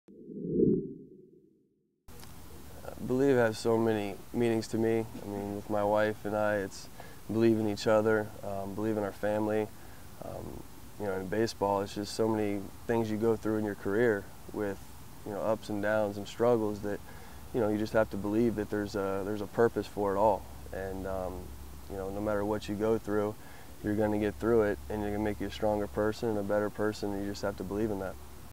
Jarrod Saltalamacchia: Gotta Believe
Red Sox Catcher Jarrod Saltalamacchia and the need to believe.